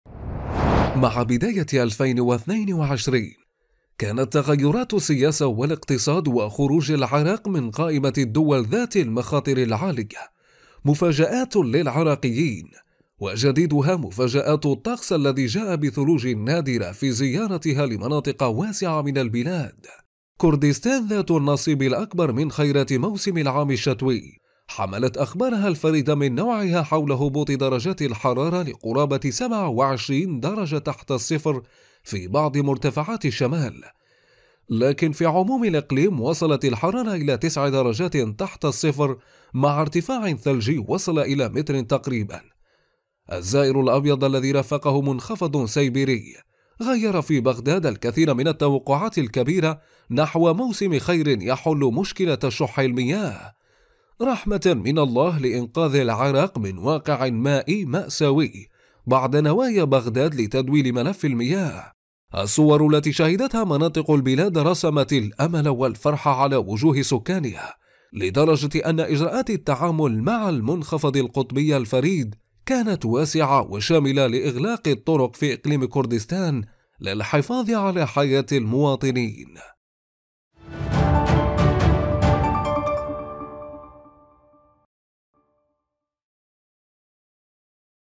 ثلوج العراق تفاجئ العالم بحالة نادرة وتحل مشكلة خطيرة | تقرير
حمل موسم الشتاء الحالي، مفاجآت غير متوقعة لم يسبق لمناطق عراقية أن شهدته كتساقط الثلوج لأول مرة فيها، وارتفاع نادر للثلوج في كردستان ودرجات متدنية جدا تحت الصفر.

موقع قناة التغيير الفضائية الالكتروني